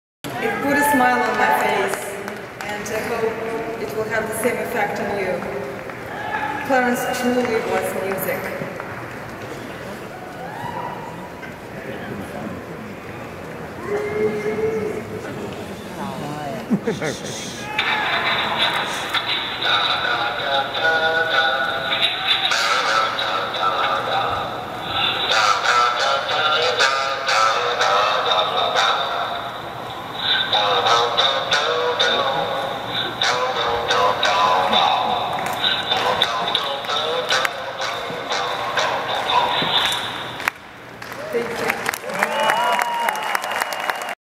Victoria Clemons at the Rock and Roll Hall of Fame, 4/11/14
Victoria Clemons plays Clarence's voice memo